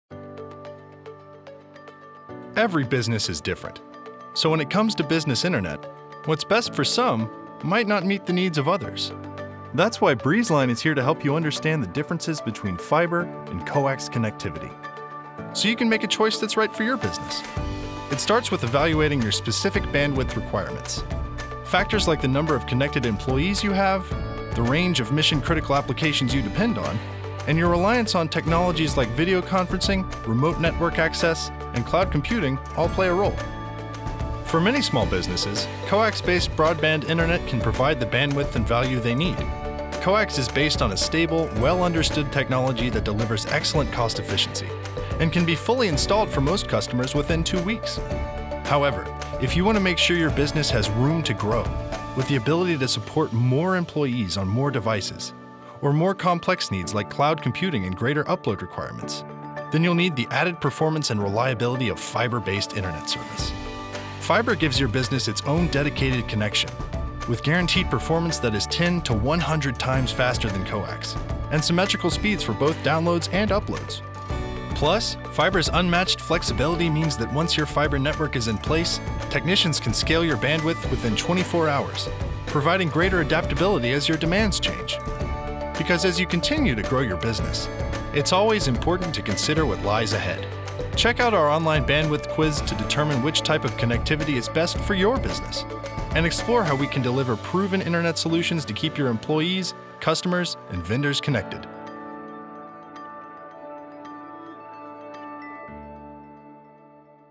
Fiber or Coax: Finding the right fit for your business
Learn the differences between Fiber Internet or coax, let Breezeline help you find the right fit for your business